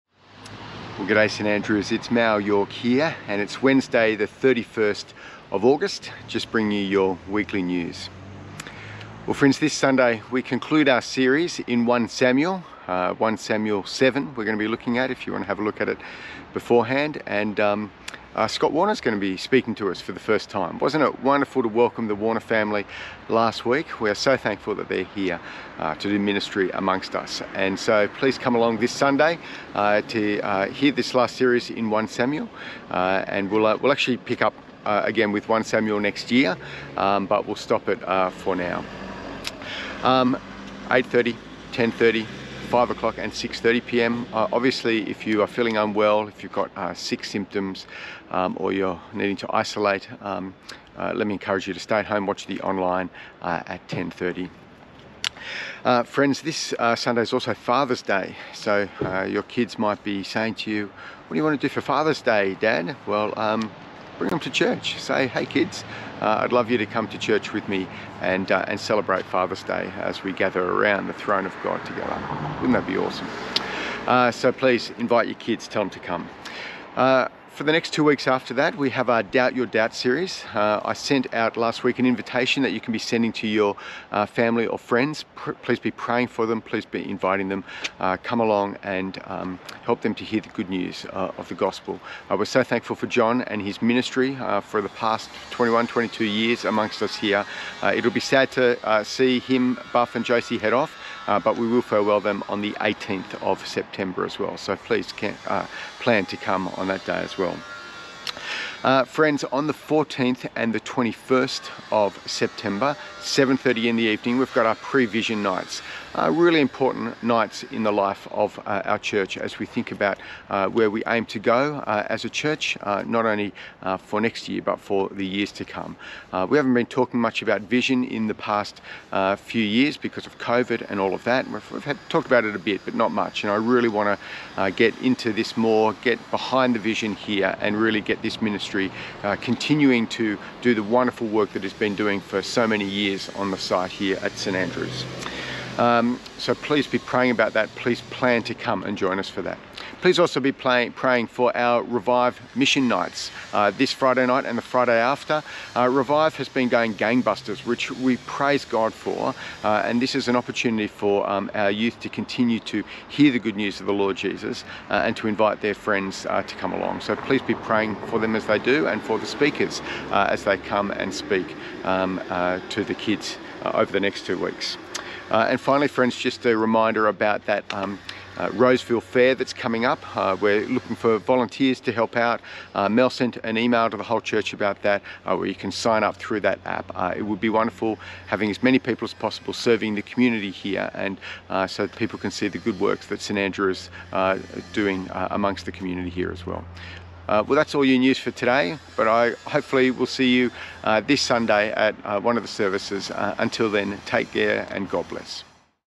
Weekly News 31 08 22
This is the weekly news for members of St Andrew's Anglican Church Roseville.